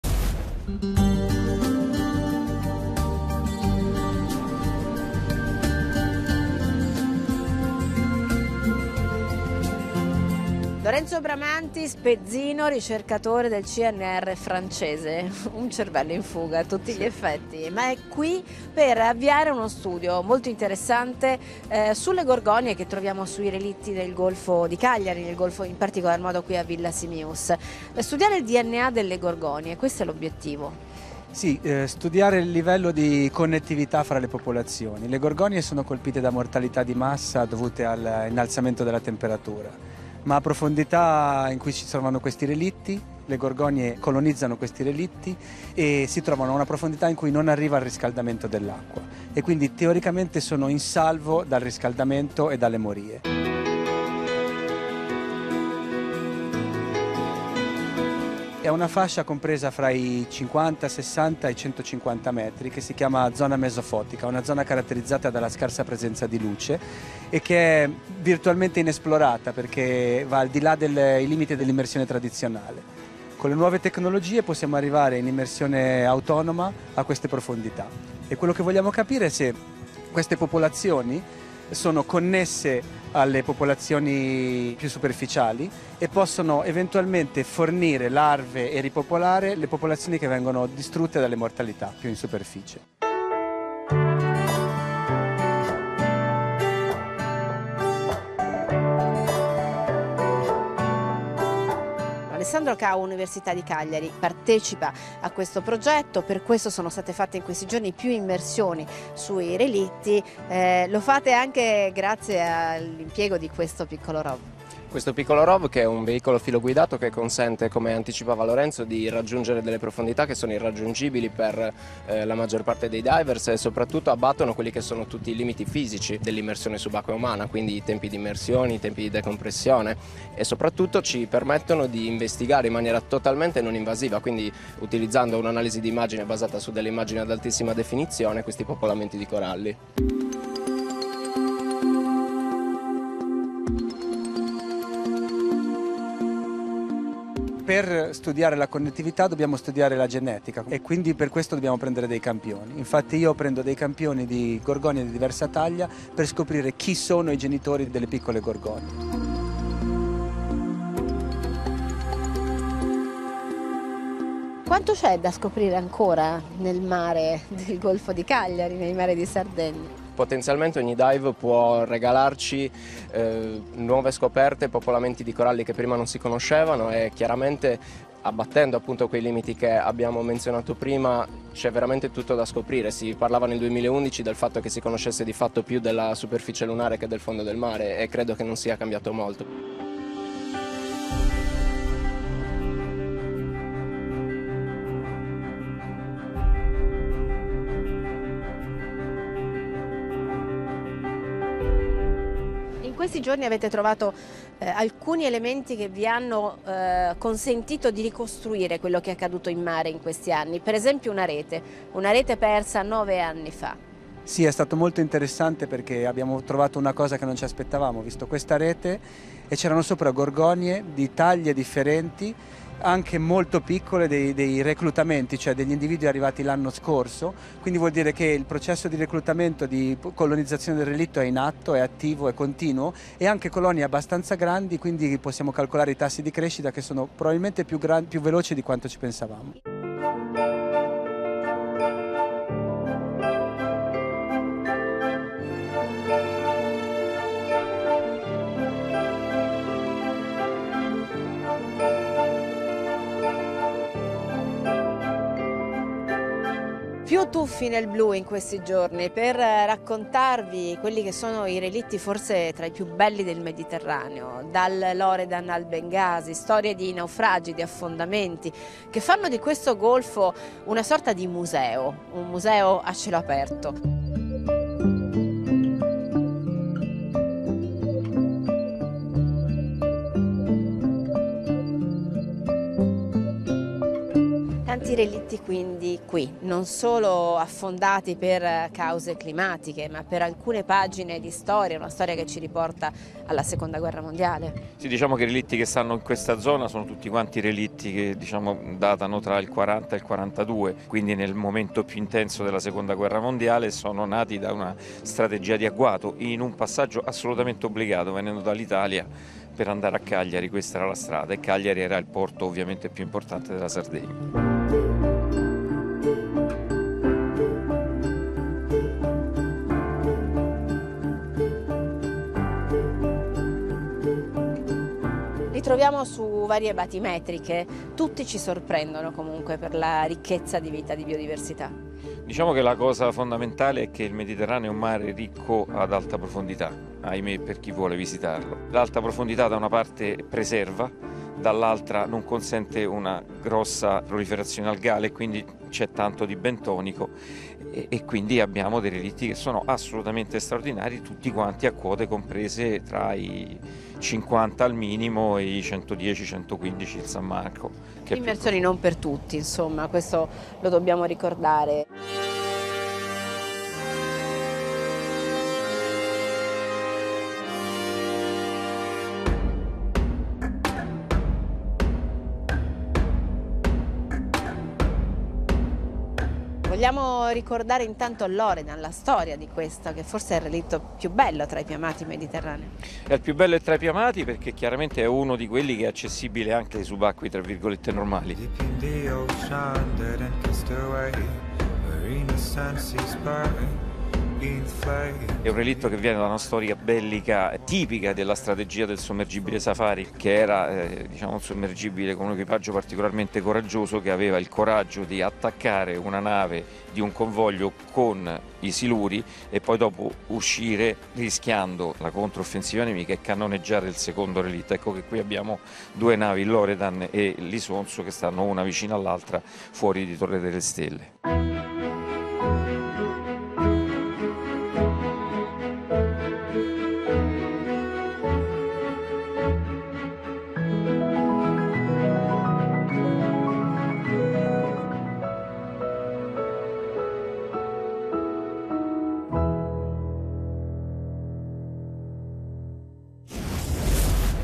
Deep gorgonians
deep wrecks as refugia fro gorgonian populations